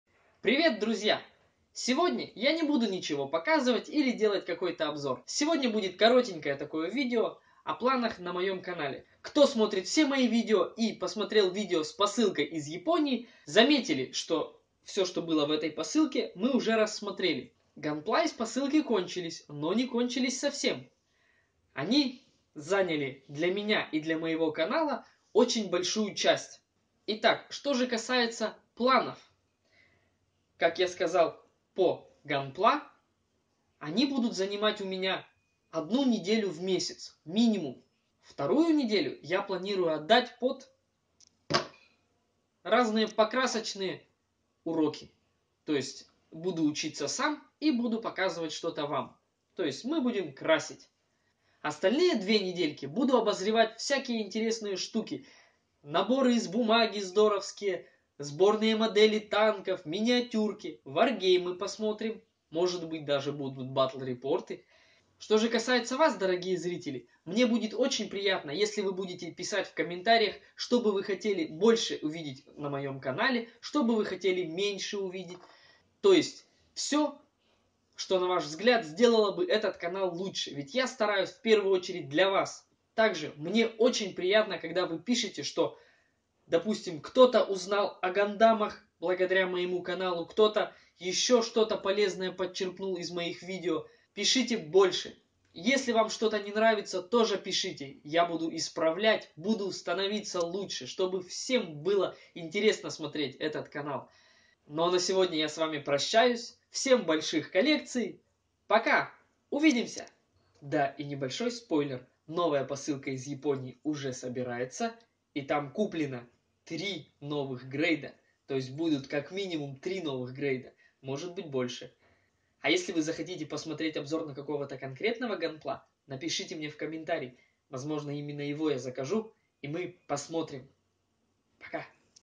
Планы по дальнейшему развитию канала.
Сорян за расфокусировку камеры.
Пока "обратной связи" у меня нет, потому как нет времени создавать и вести группу канала, можете свободно писать предложения и пожелания сюда)